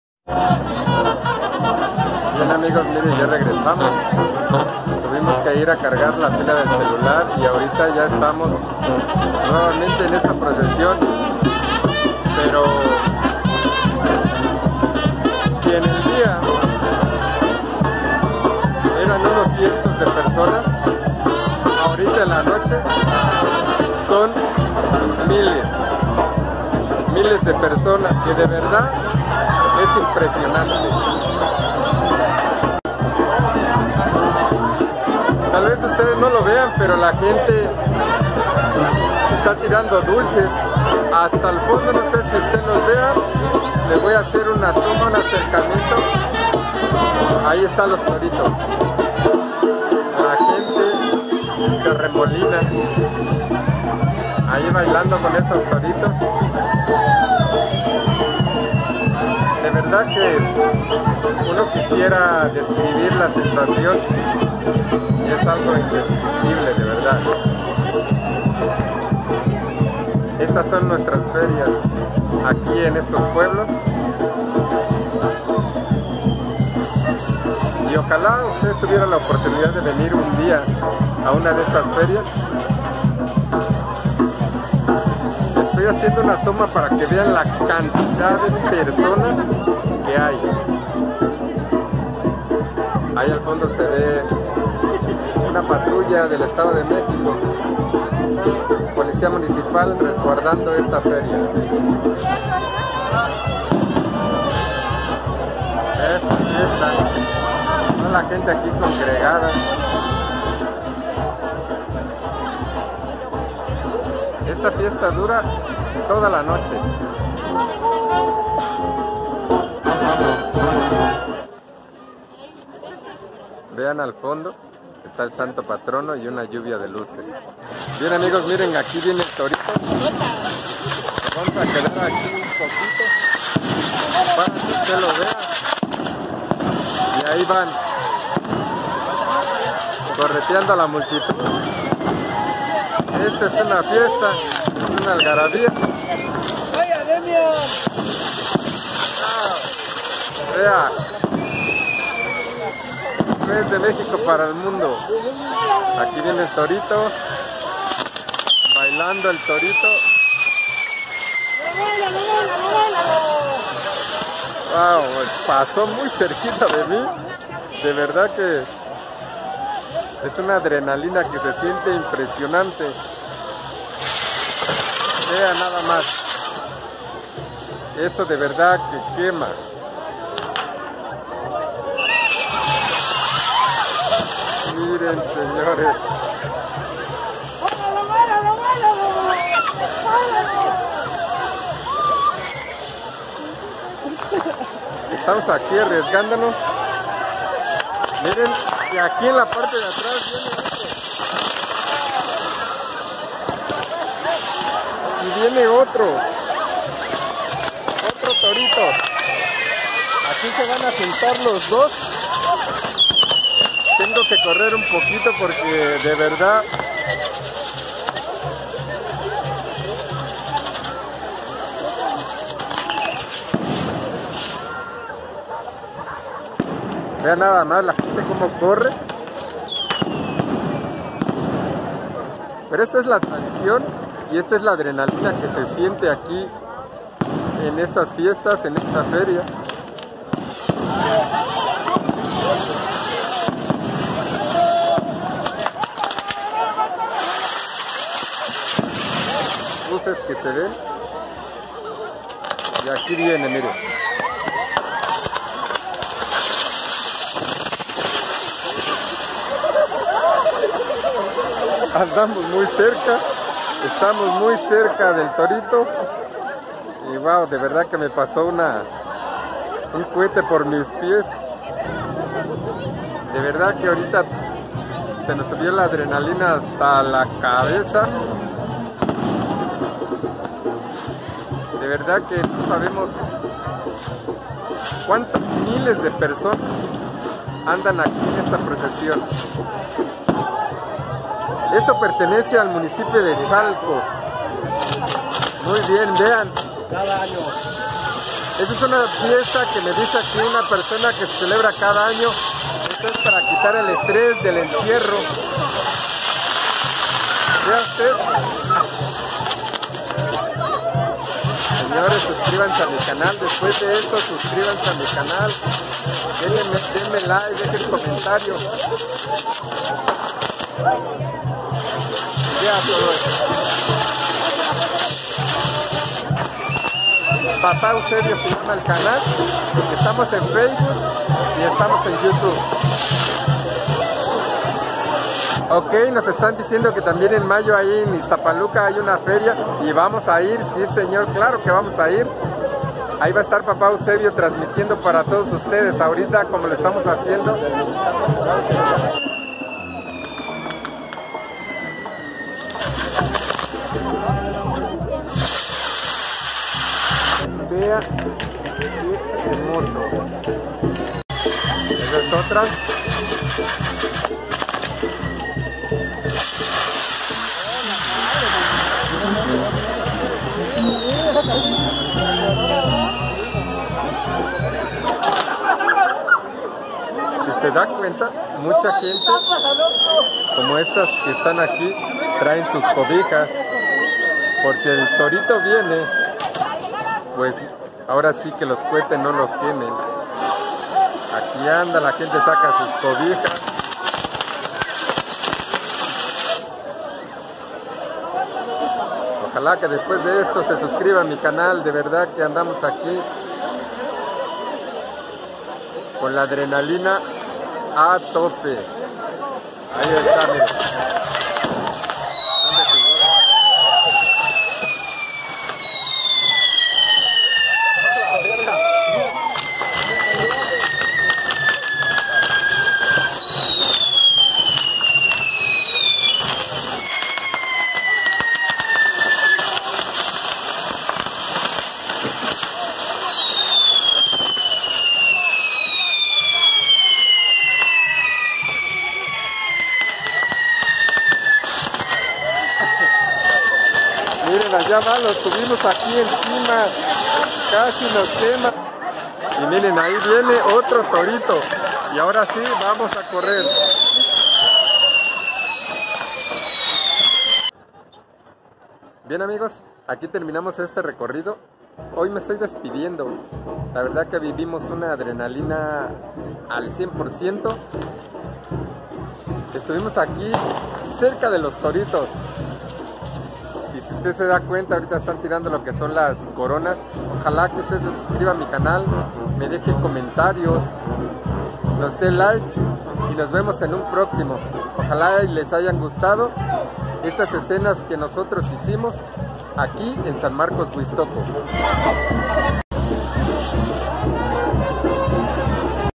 Fiesta Sn Marcos Huixtoco 2022 Papá Eusebio 😲😲
La Fiesta De Sn Marcos Huixtoco Municipio De Chalco Edo Mex Ahora En La Noche